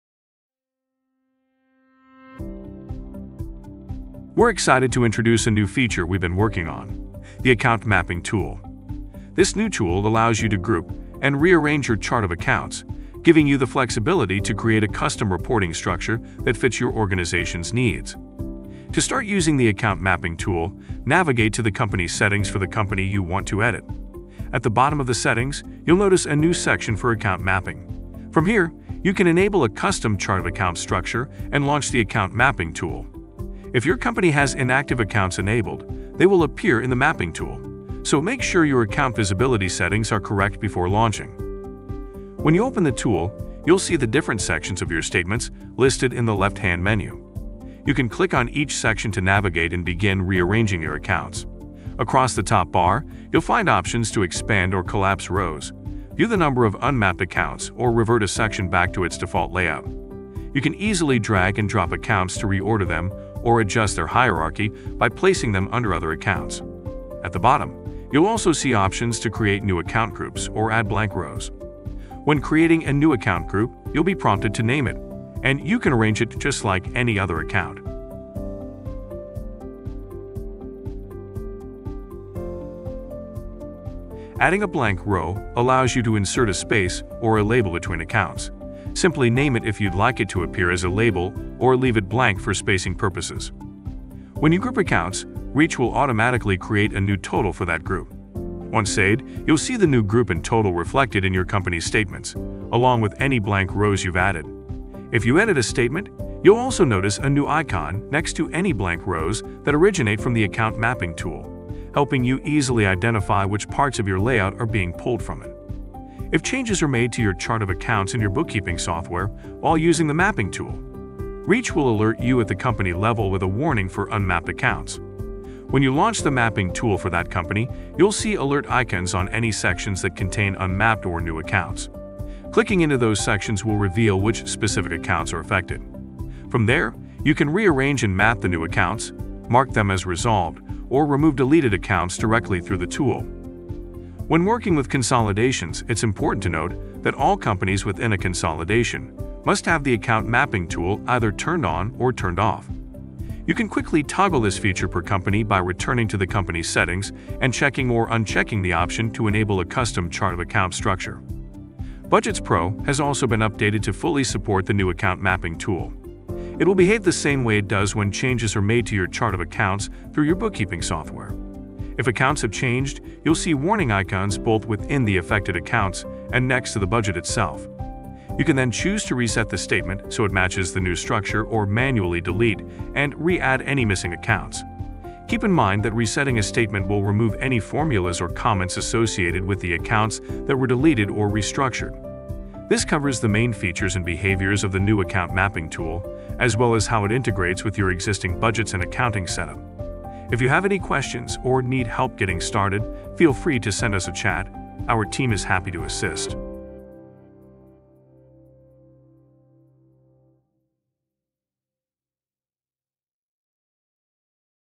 Account Mapping Tool
Your reporting just got a big upgrade! With the new Account Mapping Tool, you can easily group and rearrange your Chart of Accounts to match how you actually want to see your data. It’s flexible, intuitive, and built to make reporting your way simple. Try it now from your company settings.